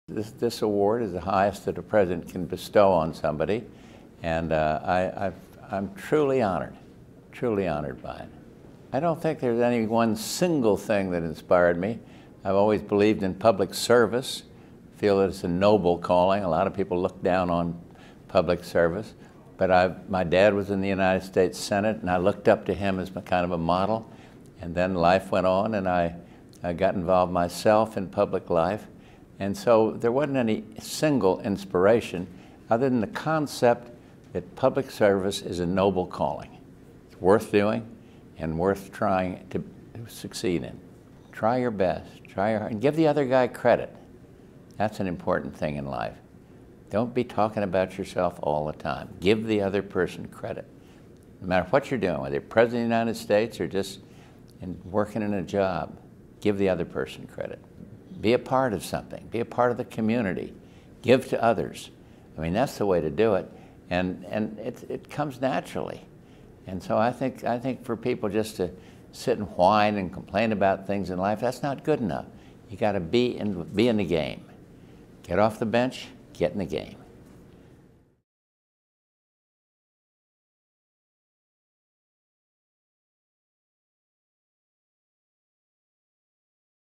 2010 Presidential Medal of Freedom Recipient - President George H.W. Bush
February 16, 2011 | 1:26

George Herbert Walker Bush was the 41st President of the United States. Prior to that, he was Ronald Reagan's Vice President, Director of Central Intelligence, Chief of the U.S. Liaison's Office to the People's Republic of China, U.S. Ambassador to the United Nations, and a Member of the House of Representatives from the 7th District of Texas. He served in the Navy during World War II. President Bush and President Clinton appeared together in television ads in 2005, encouraging aid for victims of Hurricane Katrina and the 2004 Indian Ocean tsunami.